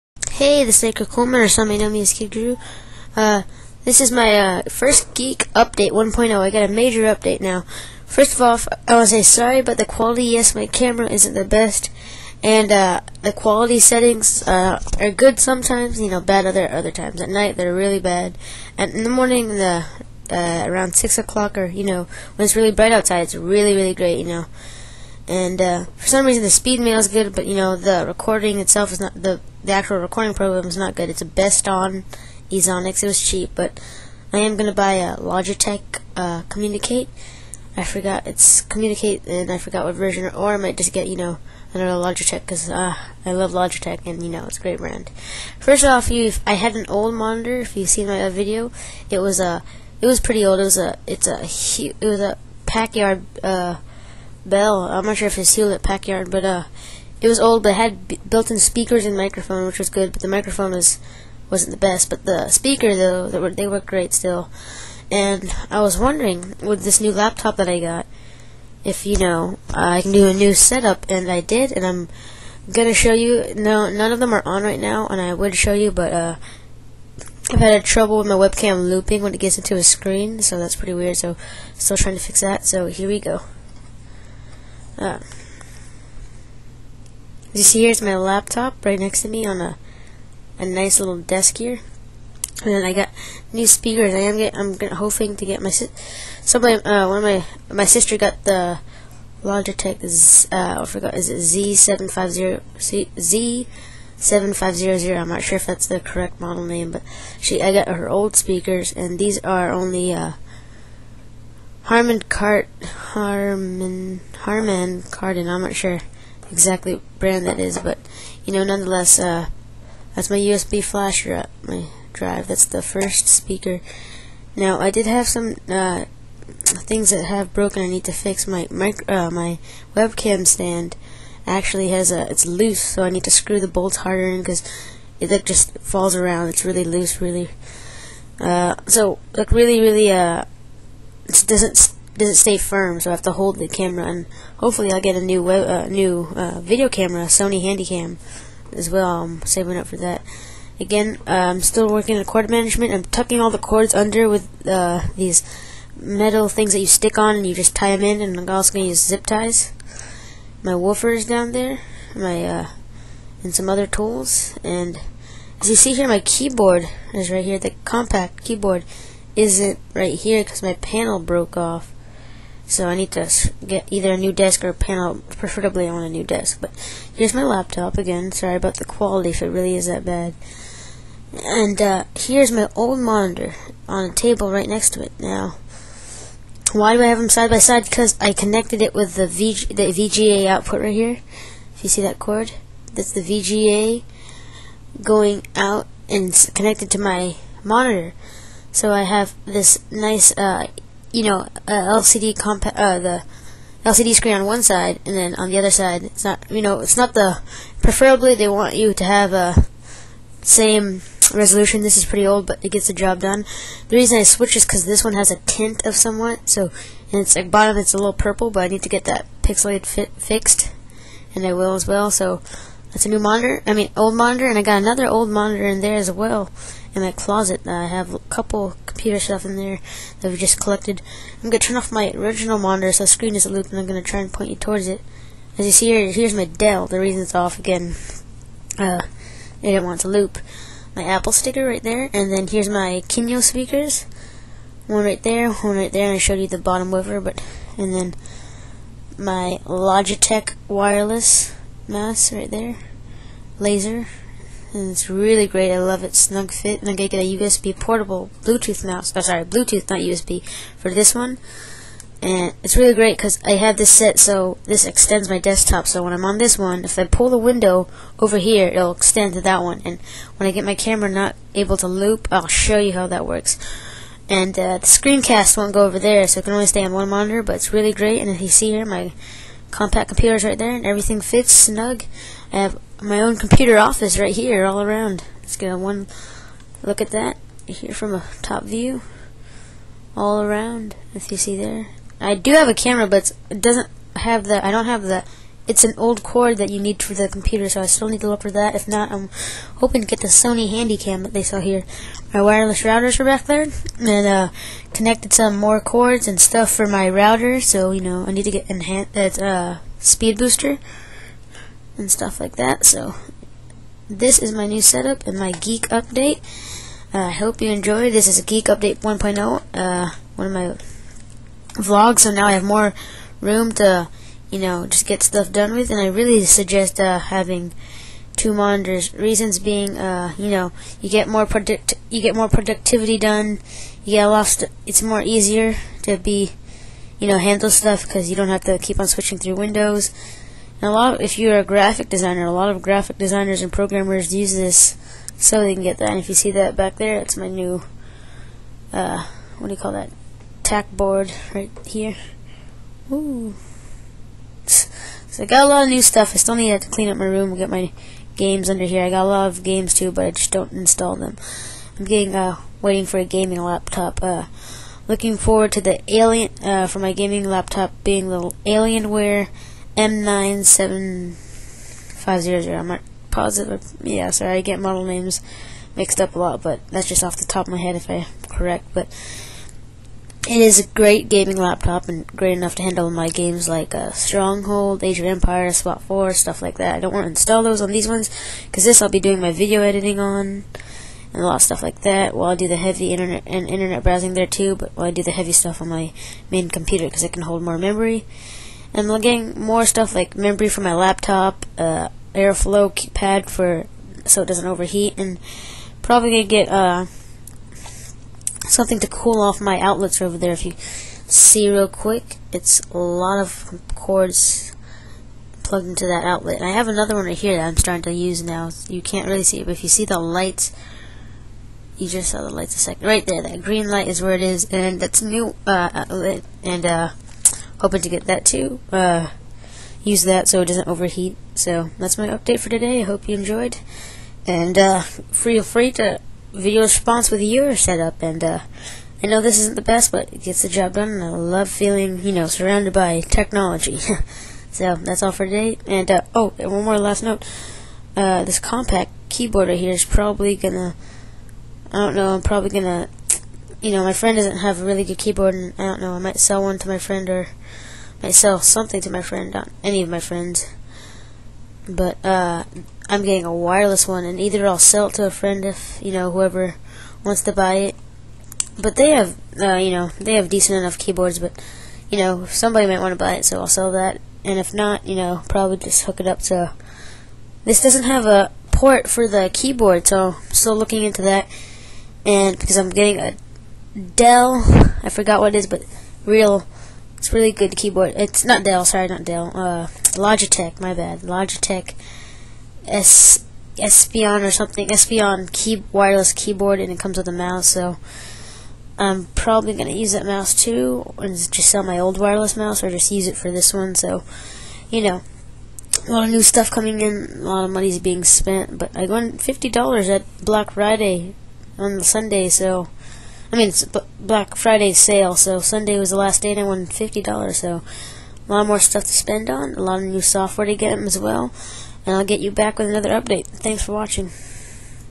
Geek Update V. 1.0
All content going forward will be uploaded there so don't miss out. Geek Update V. 1.0 My Setup with 2nd monitor sorry for quality in new update i have the new webcam that I will refilm with for a better look at the set up Enjoy